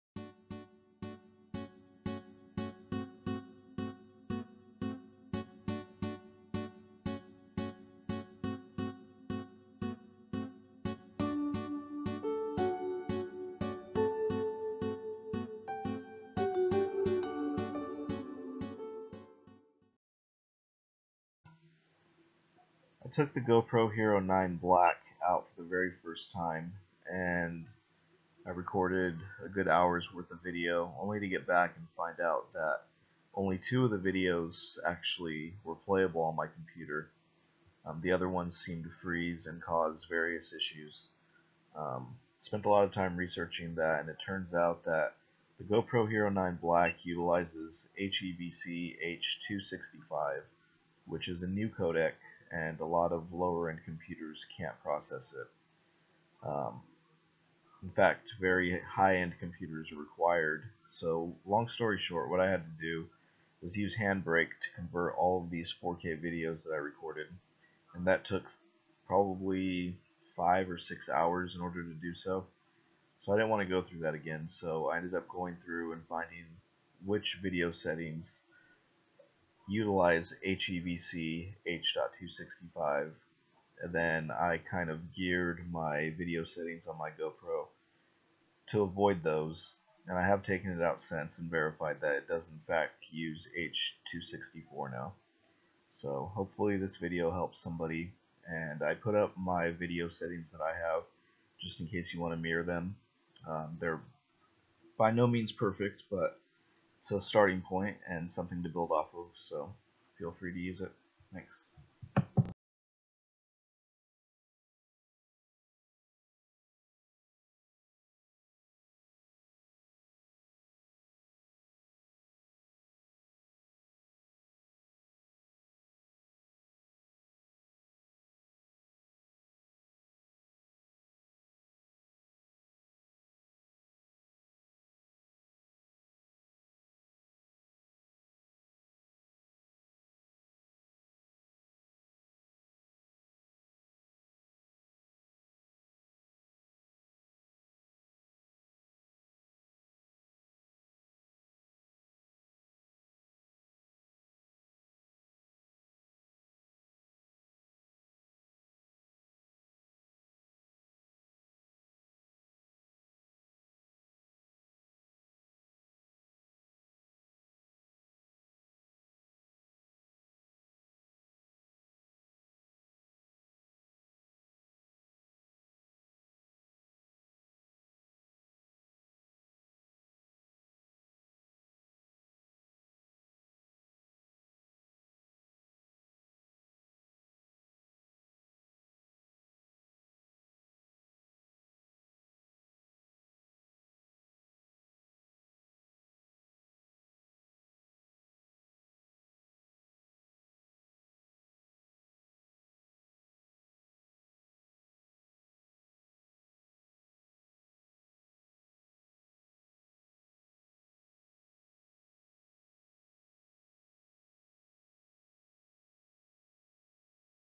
GoPro Hero 9 Black - How To Avoid HEVC H.265 And Use H.264
I took out my new GoPro Hero 9 Black and recorded a few long videos only to have the headache of having to convert multiple 4k videos with Handbreak (and as a result, lowering the 4k down to 1080p in the process). That seemed to be a waste of the GoPro's quality and I figured out what quality/fps to avoid so the GoPro wouldn't use HEVC H.265 and would always use H.264. Now I can take my videos into OpenShot Video Editor, for example, and when done, export 4k 30fps video to upload to YouTube and retain all the quality.